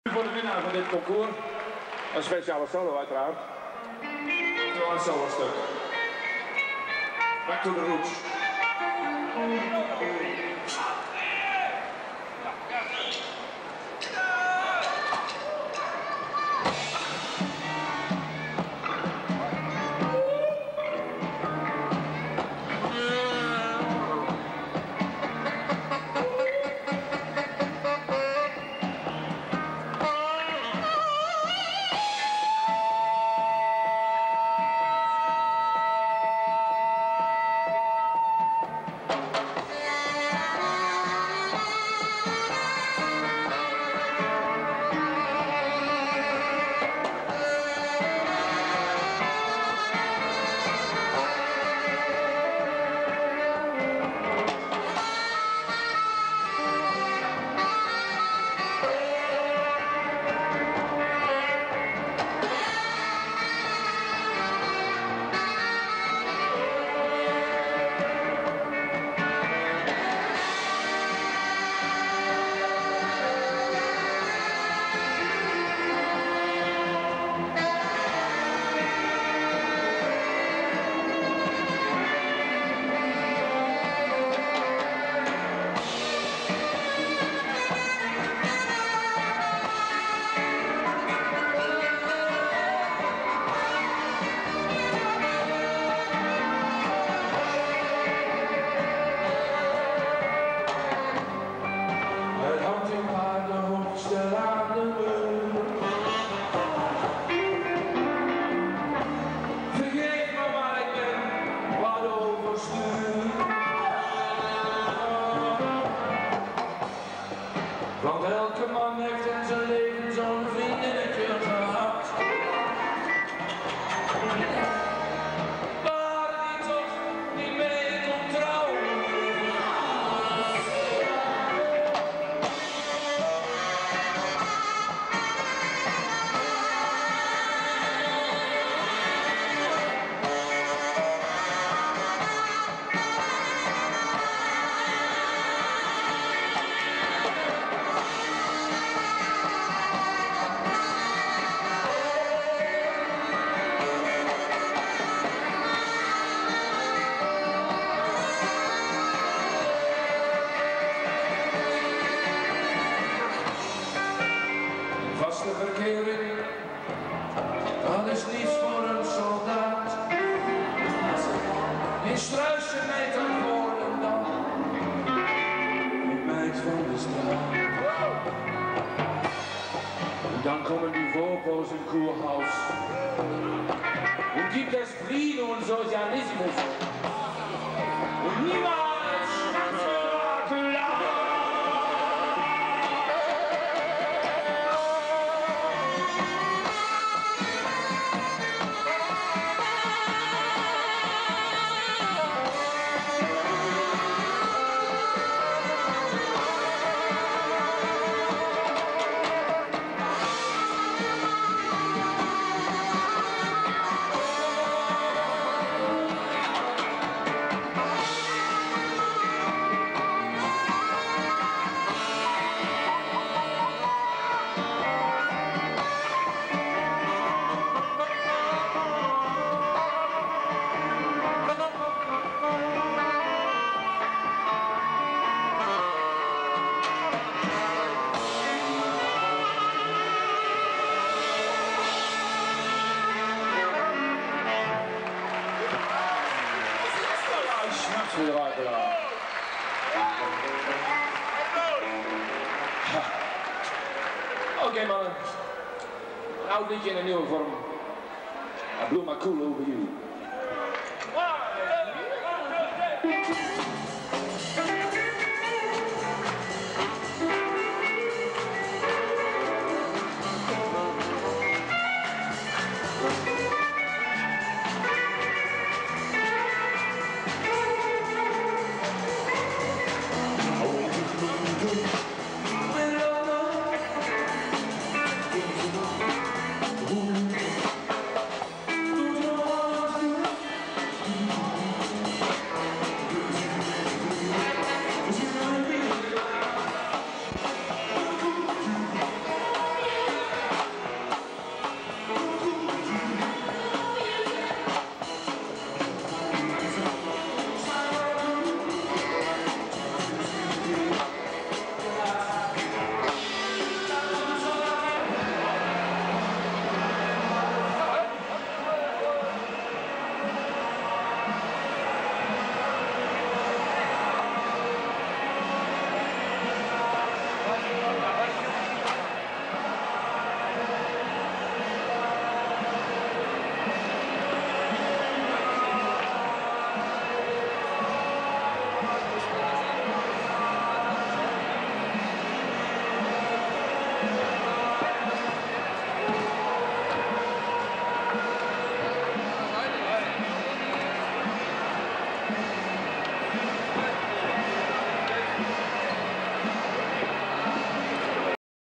Appie Alberts c.s. in de IJshal in Groningen op 25-8-1985 bij Sterren in het Park
Appie Alberts c.s., bestaande uit, Appie Alberts op sax en zang, Rudy Lentze op gitaar, Wessel Babtist op drums, Harry van Lier op basgitaar, Hans Dulfer op sax, Boris van der Lek op sax, Rinus Groeneveld op sax en de Bombitas op zang en dans, in de IJshal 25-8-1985 tijdens Sterren in het Park, Indoors vanwege het weer. Camera door Bernard of Leen.